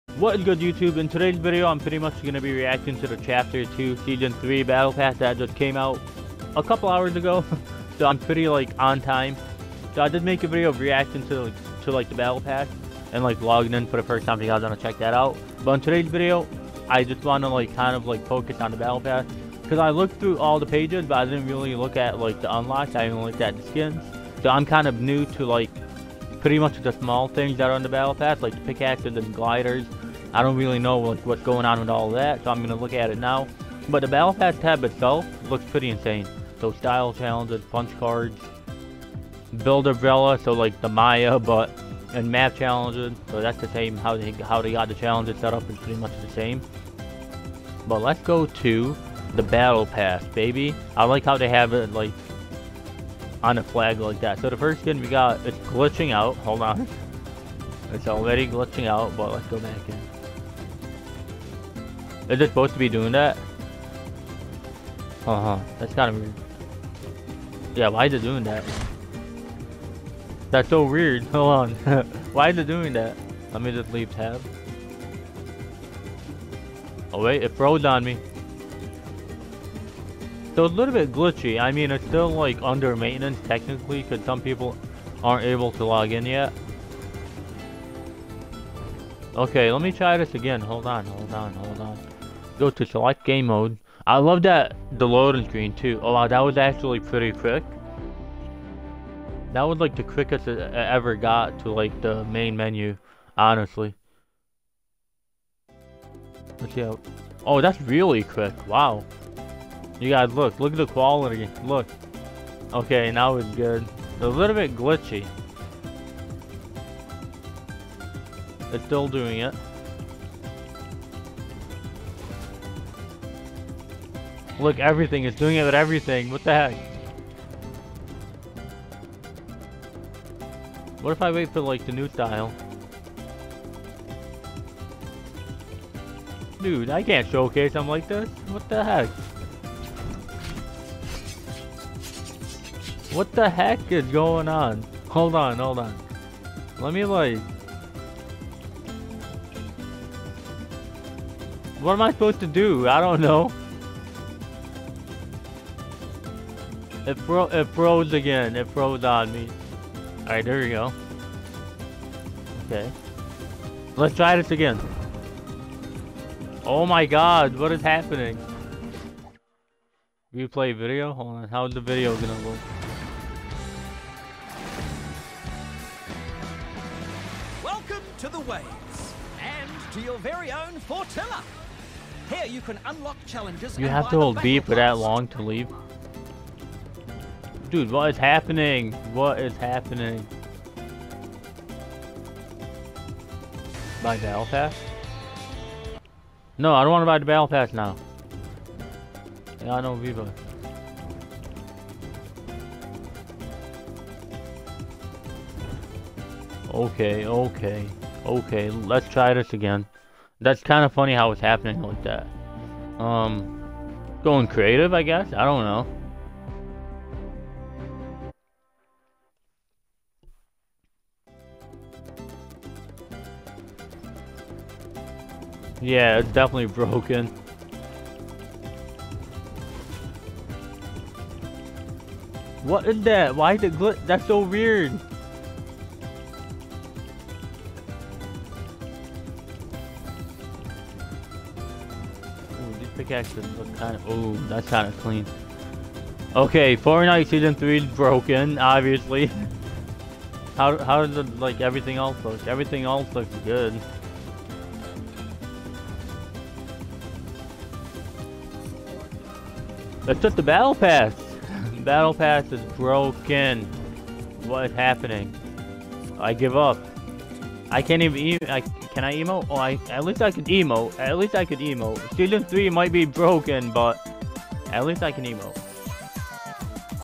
Season 3 is BROKEN
Season 3 looks amazing when glitched, can't imagine what it will look like when it gets fixed!

Battlepass review and showcase video coming soon as soon as Season 3 acts normal again.